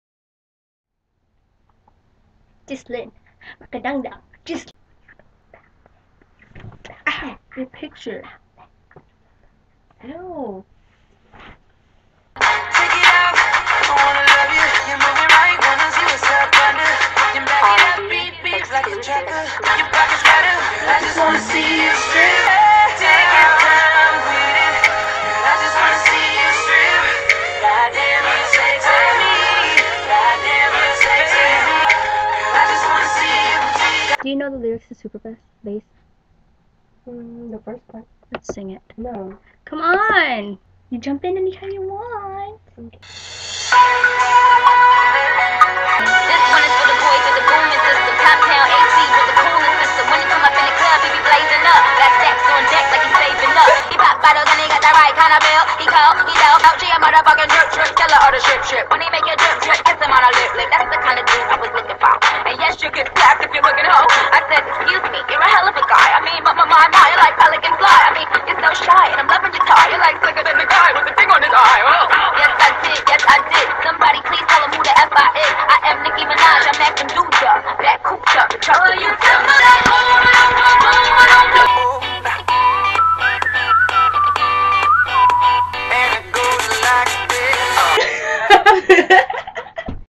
A Day with Jeslynn & Cory
I made this last year around September (2011)  I think, lol. My cousin and I being silly! Mostly me though ^^